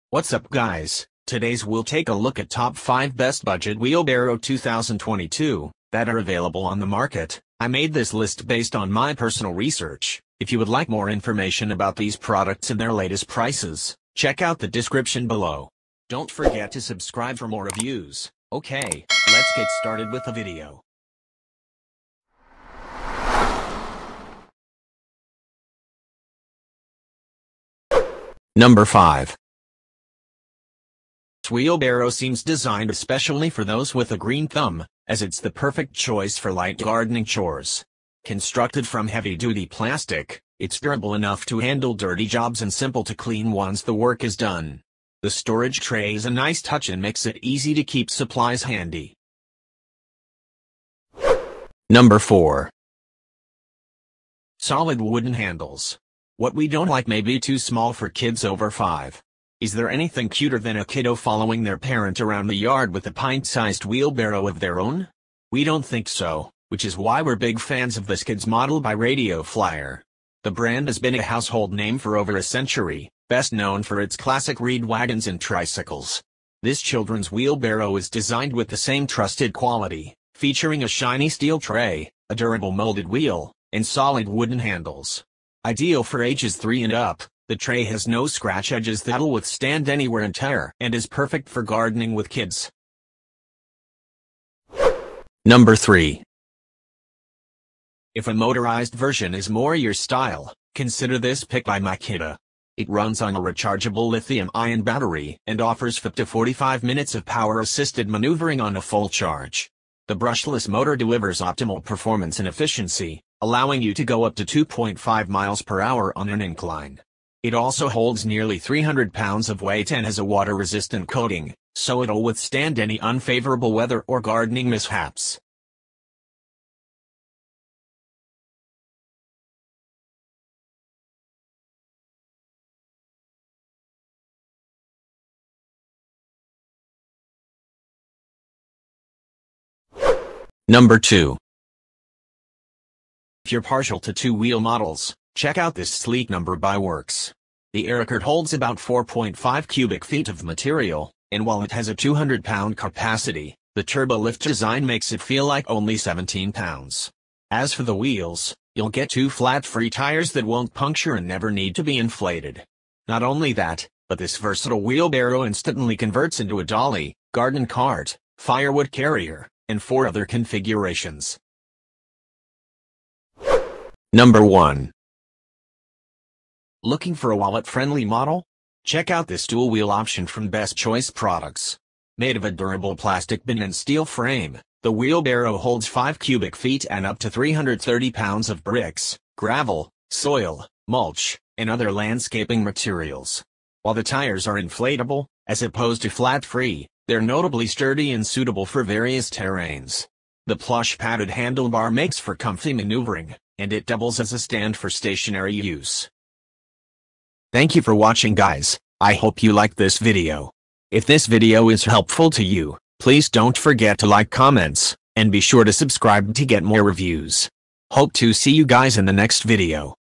Top 5 Best Budget Wheelbarrow Reviews in 2024
In This Video, I Listed the Top 5 Best Budget Wheelbarrow Reviews in 2024, You Can Check Out the Latest Price or Purchase in the Description Below!

05.Garden Star Dual Wheel, Poly Tray Yard Rover Wheelbarrow

04.Heavy Duty Poly Yard Garden Steel Dump Utility Wheelbarrow

03.WORX WG050 Aerocart 8-in-1 Yard Cart / Wheelbarrow

02.Brushless Power-Assisted Wheelbarrow

01.SuperHandy Wheelbarrow